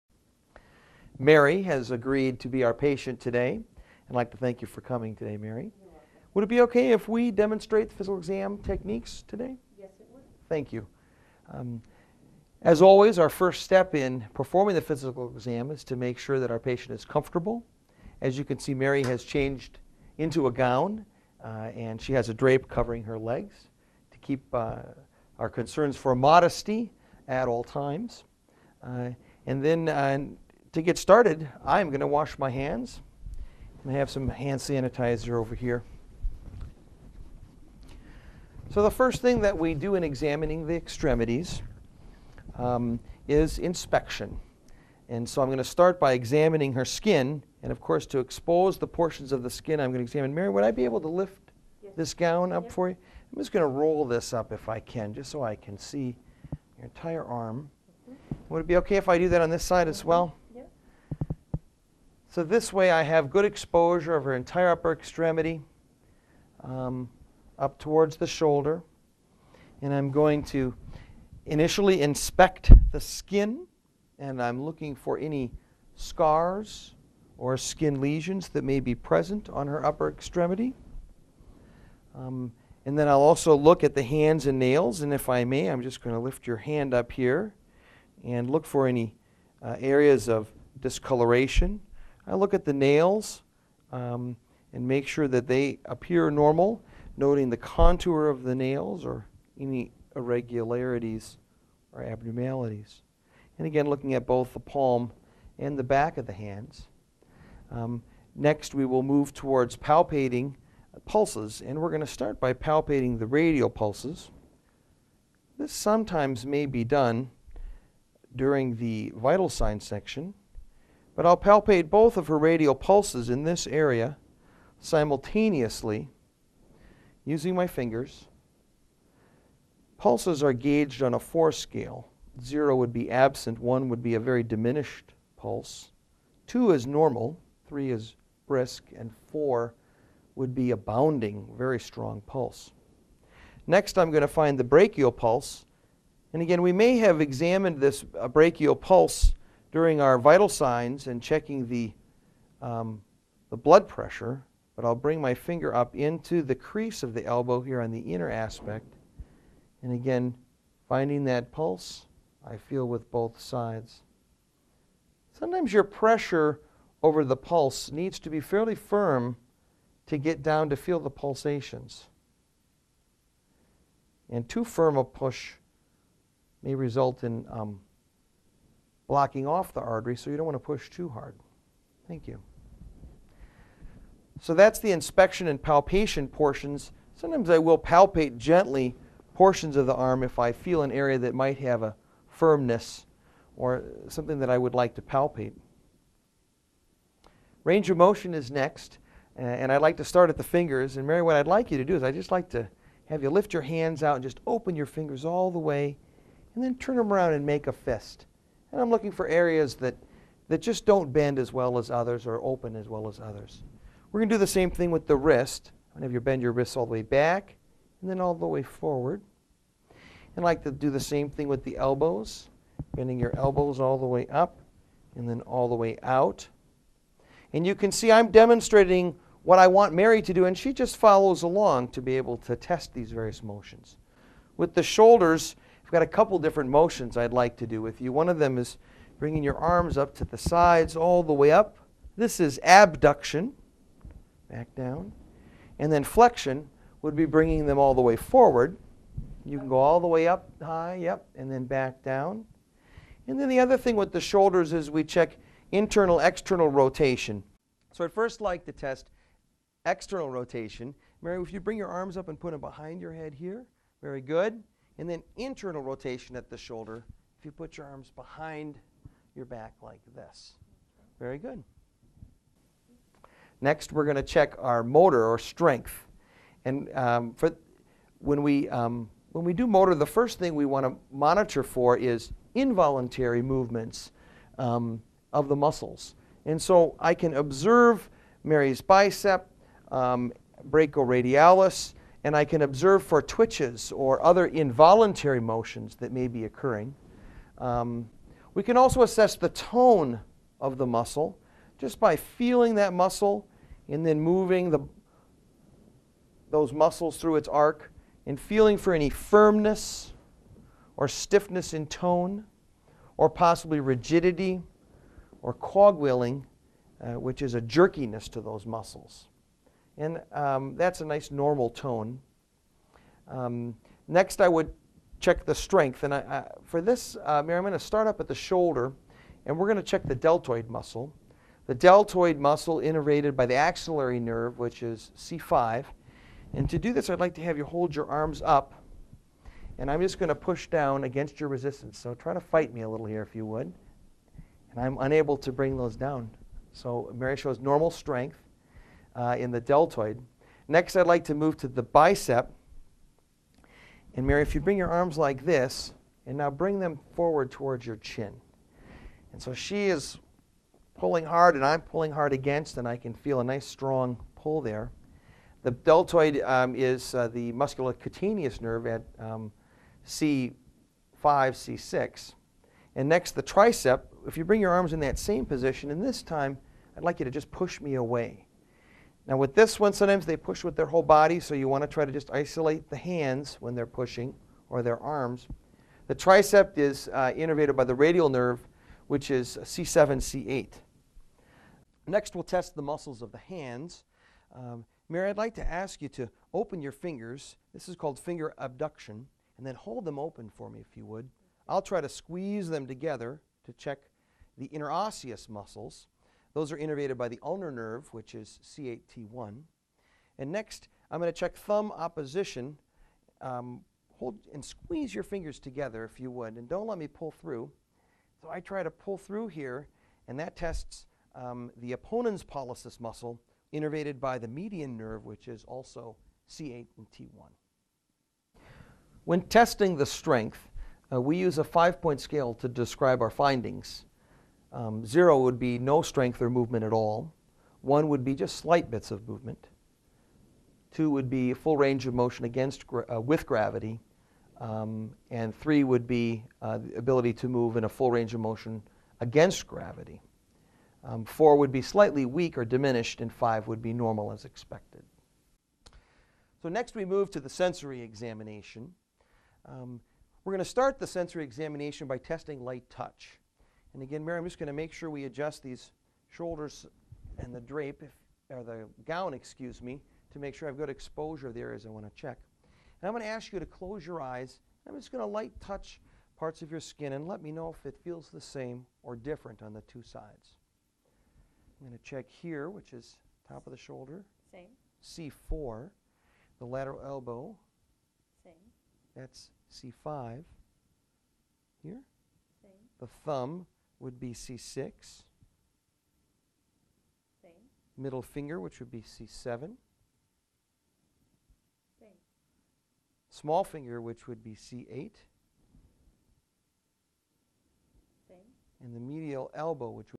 06a.Physical Exam -Upper & Lower Extremities -Part 1/4
Physical Examination. University of Wisconsin School of Medicine and Public Health.
Video 06a/19 (Video 06 is divided in 4 parts)